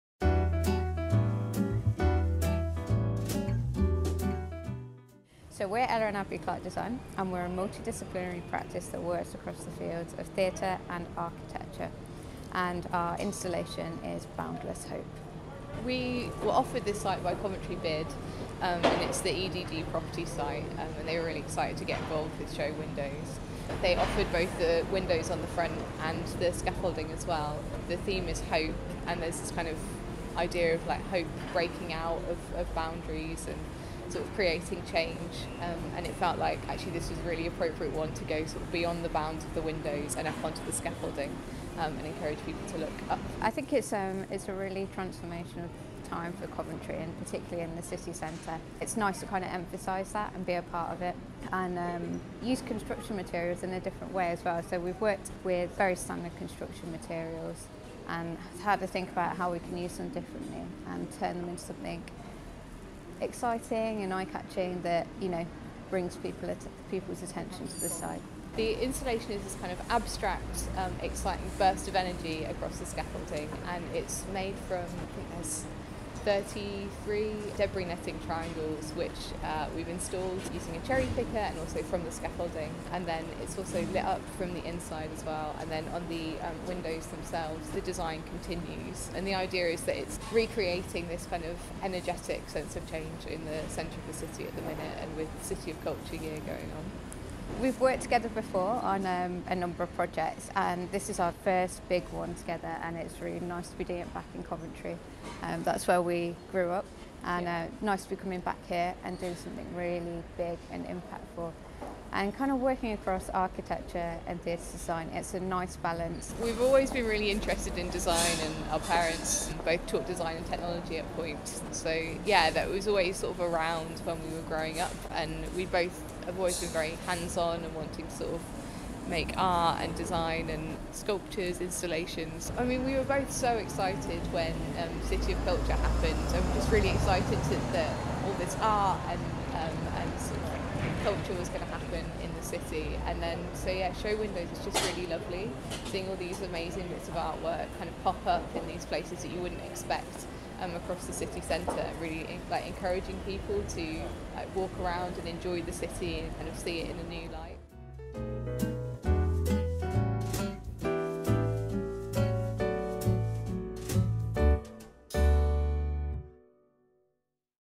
The Show Windows: Boundless Hope in The Burges
Our biggest Show Windows display to date, Ella and Abby Clarke Design's "Boundless Hope" spills out of the windows and across the scaffolding of the EDG Property site at the top of The Burges.

Brought up in Coventry, sisters Abby and Ella combine their respective practices in theatre design and architecture for their collaborative work, and received the commission for Boundless Hope through RIBA West Midlands. Created using construction materials, this bright, playful piece invites passers-by to take notice and view the urban surroundings in a new light.